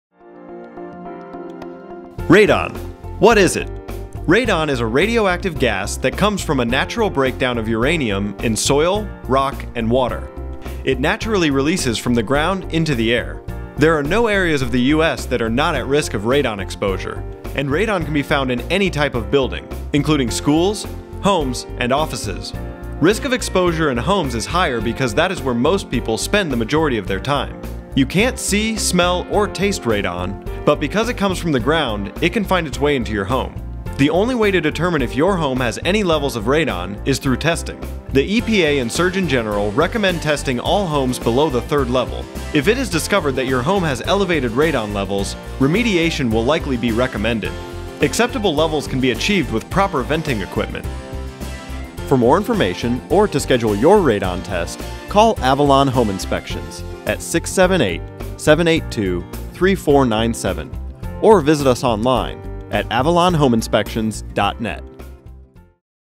Avalon Home Inspections | What is Radon?
This video provides an overview of what Radon is and the importance of performing radon testing.